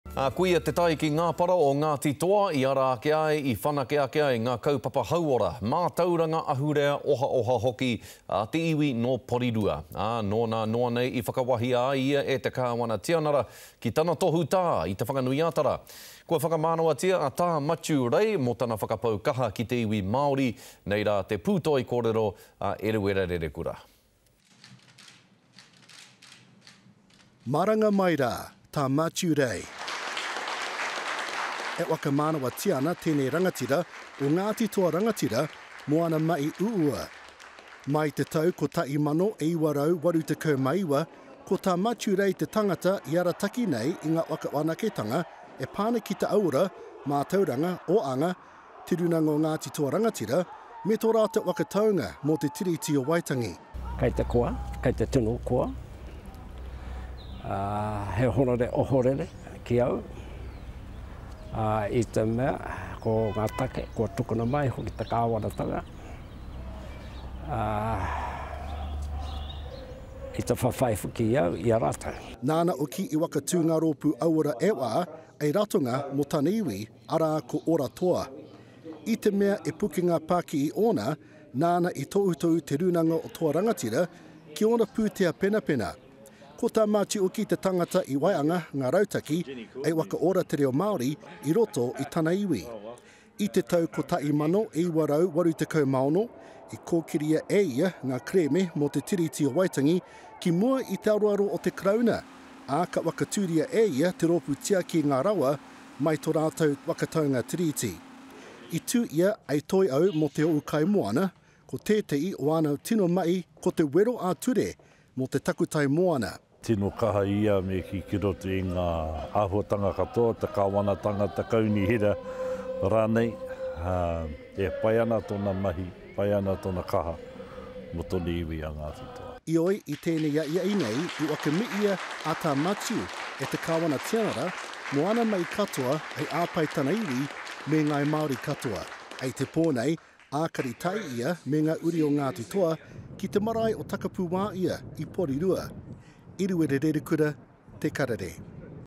Ngāti Toa leader Matiu Rei knighted
The Ngāti Toa leader behind the health, education, culture and economic development of the Porirua-based iwi was knighted at a special ceremony at government house in Wellington today. Tā Matiu Rei was honoured in this year's Queen's Birthday Honours for his services to Māori. Eruera Rerekura looks back at some of his achievements.